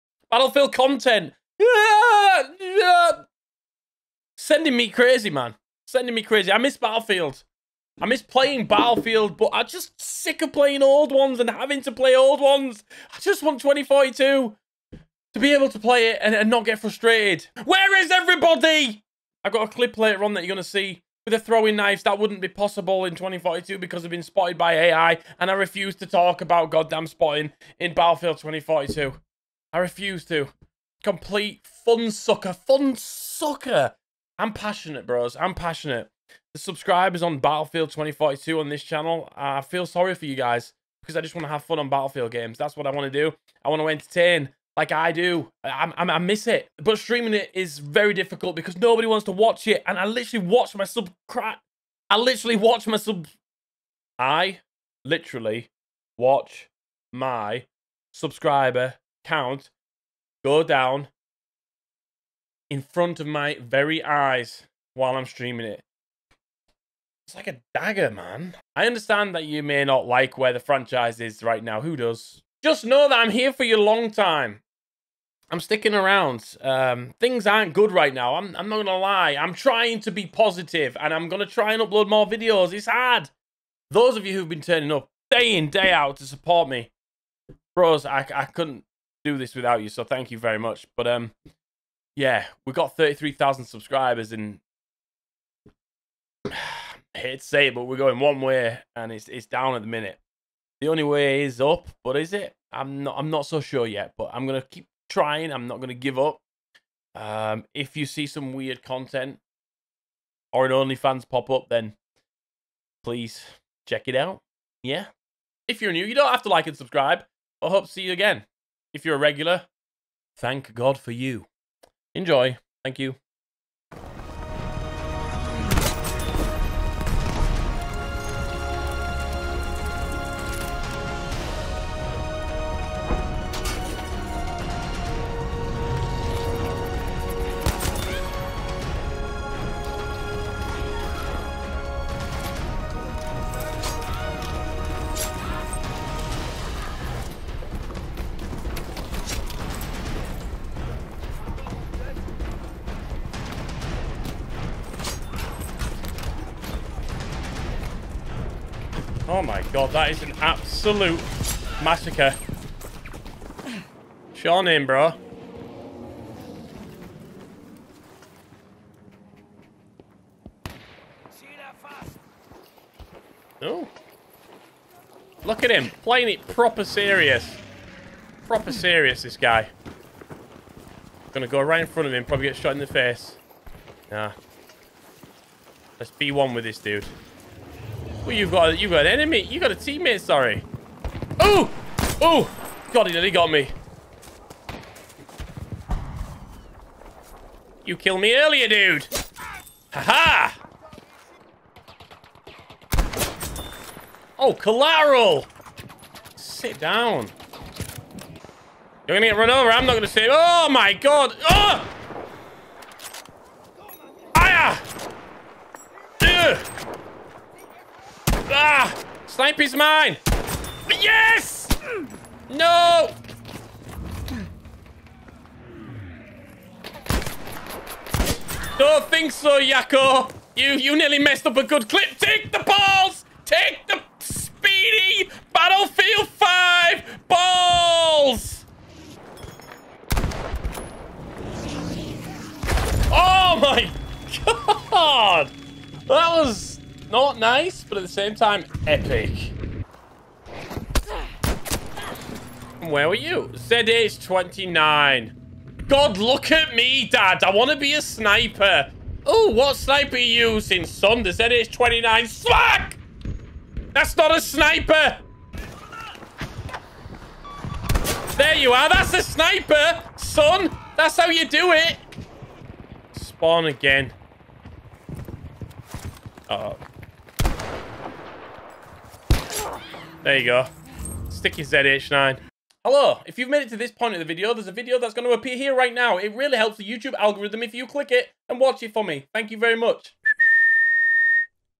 Will Battlefield End My Channel?
The Battlefield Franchise has been my love for so long. I want it to continue but at this moment in time its hurting my channel. Please DICE fix the issues with 2042. Even then, will players return to the game?